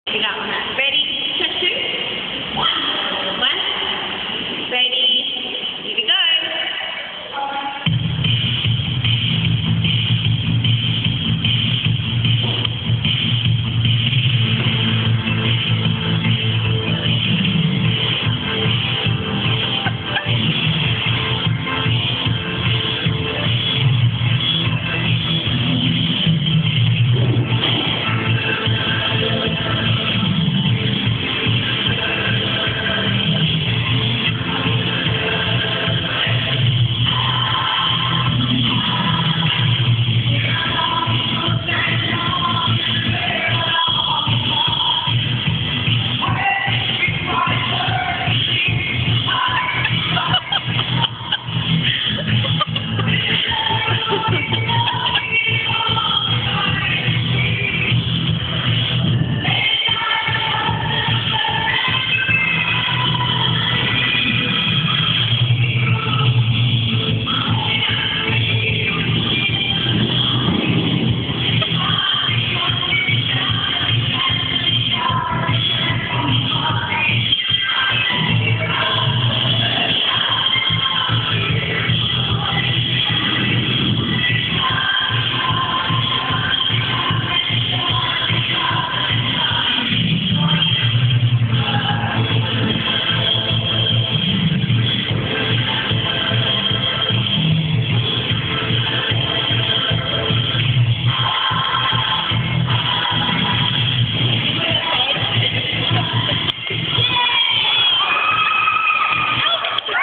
Robot? dance?...
back in the day when teachers were absent from a dance class. i really didnt take dance so seriously... lol was fun though.

bad quality